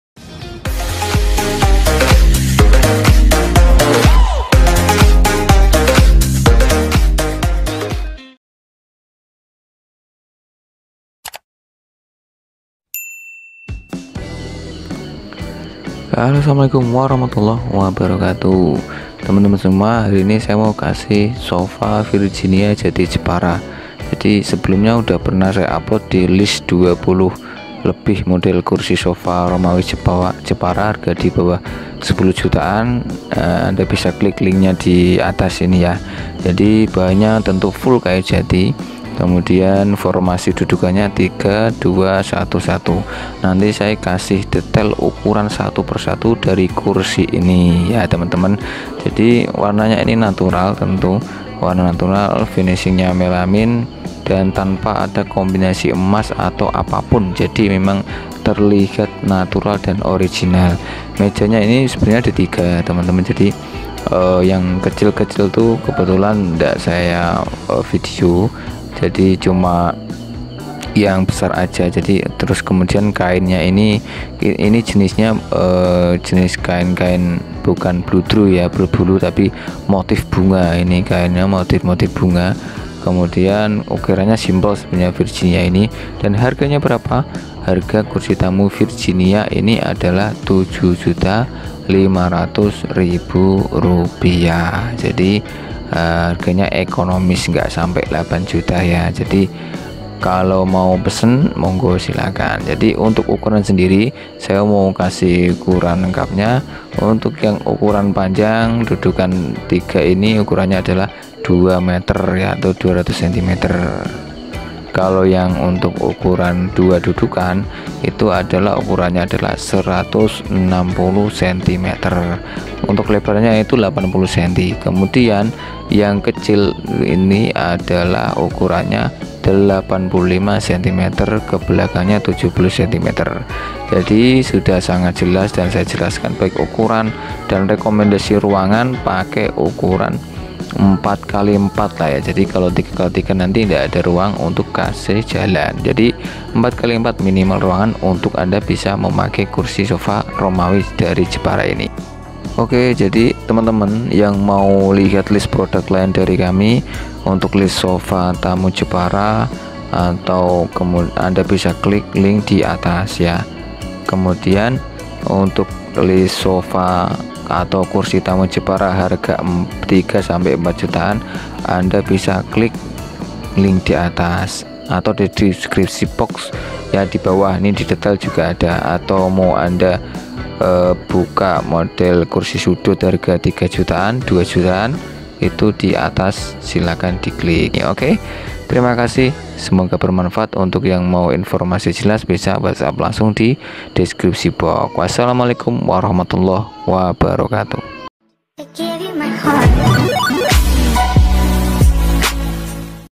SOFA TAMU MURAH JATI NATURAL
Kursi Sofa Tamu Mewah Harga Murah gak ada 8 juta, anda sudah bisa mendapatkan model kursi sofa romawi warna natural. semua detail dan spesifikasi sudah kami sampaikan dalam video.